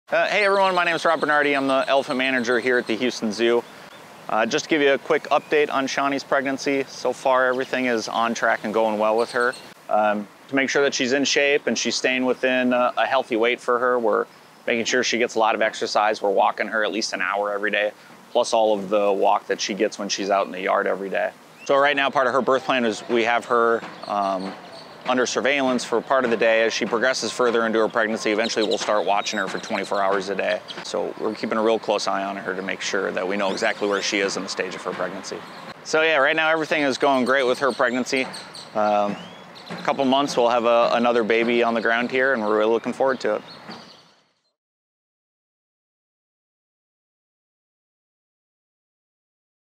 Shanti's Pregnancy Update
We’re expecting a baby elephant to join our herd this spring! Check out the latest updates of our soon-to-be-mom, Shanti.